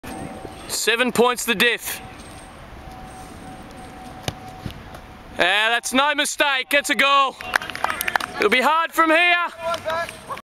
Kanga Footy - Bibs Only Vs Such Is Life, Round 5 2011
Full Forward Michael Stockley gives Bibs Only some breathing space.